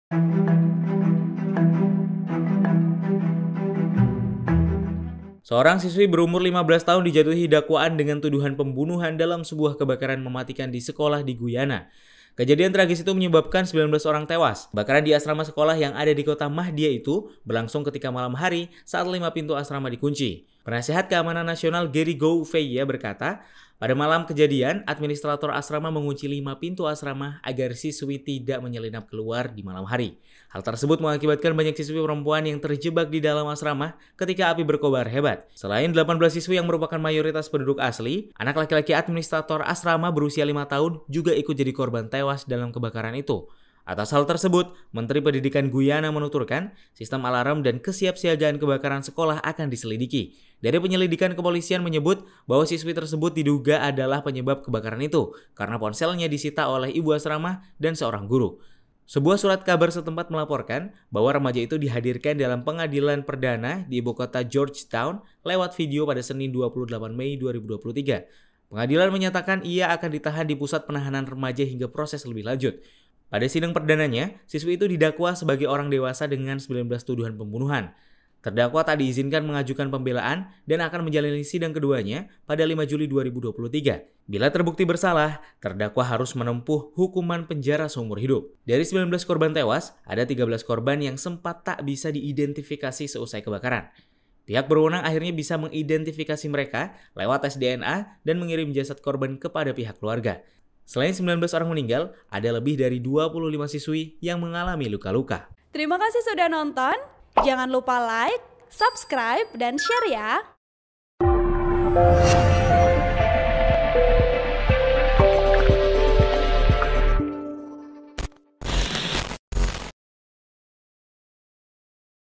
Kesal Ponselnya Disita Guru, Seorang Siswi Membakar Asrama Sekolah sebabkan 19 Orang Tewas
TRIBUN-VIDEO.COM - Seorang siswi berumur 15 tahun dijatuhi dakwaan dengan tuduhan pembunuhan dalam sebuah kebakaran mematikan di sekolah di Guyana.

Kejadian tragis itu menyebabkan 19 orang tewas.

Kebakaran di asrama sekolah yang ada di Kota Mahdia itu berlangsung ketika malam hari saat lima pintu asrama dikunci.

Penasihat Keamanan Nasional Gerry Gouveia berkata, pada malam kejadian, administrator asrama mengunci lima pintu asrama agar para siswi tidak menyelinap keluar di malam hari.

Hal tersebut mengakibatkan banyak siswi perempuan yang terjebak di dalam asrama ketika api berkobar hebat.

Selain 18 siswi yang merupakan mayoritas penduduk asli, anak laki-laki administrator asrama berusia lima tahun juga ikut jadi korban tewas dalam kebakaran itu.

Atas hal tersebut, Menteri Pendidikan Guyana menuturkan, sistem alarm dan kesiapsiagaan kebakaran sekolah akan diselidiki.

Dari penyelidikan kepolisian menyebut bahwa siswi tersebut diduga adalah menyebabkan kebakaran itu, karena ponselnya disita oleh ibu asrama dan seorang guru, dikutip dari Aljazeera.

Sebuah surat kabar setempat melaporkan, bahwa remaja itu dihadirkan dalam pengadilan perdana di ibu kota Georgetown lewat video, Senin (28/5/2023).

Pengadilan menyatakan ia akan ditahan di pusat penahanan remaja hingga proses lebih lanjut.

Pada sidang perdananya, siswi itu didakwa sebagai orang dewasa dengan 19 tuduhan pembunuhan.

Terdakwa tak diizinkan mengajukan pembelaan dan akan menjalani sidang keduanya pada 5 Juli 2023.

Bila terbukti bersalah, terdakwa harus menempuh hukuman penjara seumur hidup.

Dari 19 korban tewas, ada 13 korban yang sempat tak bisa diidentifikasi seusai kebakaran.

Pihak berwenang akhirnya bisa mengidentifikasi mereka lewat tes DNA dan mengirim jasad korban kepada pihak keluarga.

Selain 19 orang meninggal, ada lebih dari 25 siswi yang mengalami luka-luka.

(TribunVideo.com/Kompas.com)

Artikel ini telah tayang di Kompas.com dengan judul "Siswi Bakar Asrama Sekolah karena Ponselnya Disita Guru, 19 Orang Tewas", Klik untuk baca
Penulis : Aditya Priyatna Darmawan
Editor : Farid Firdaus